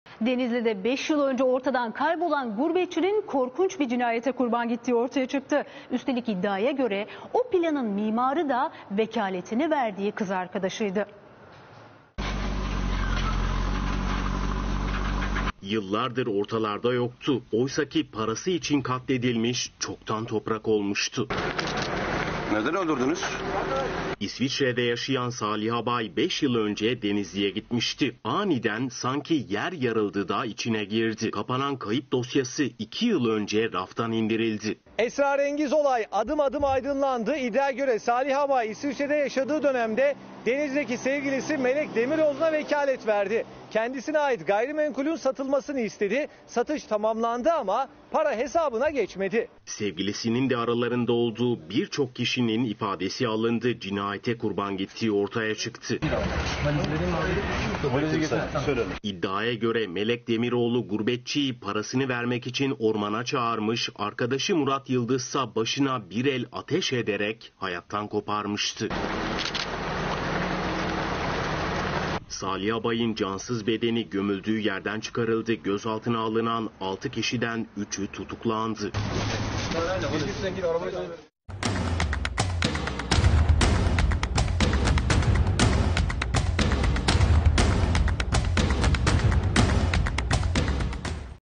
5 yıllık gizem aydınlandı!
DENİZLİ'DE 5 YIL ÖNCE ORTADAN KAYBOLAN GURBETÇİNİN KORKUNÇ BİR CİNAYETE KURBAN GİTTİĞİ ORTAYA ÇIKTI, ÜSTELİK İDDİAYA GÖRE O PLANIN MİMARI DA VEKALETİNİ VERDİĞİ KIZ ARKADAŞIYDI.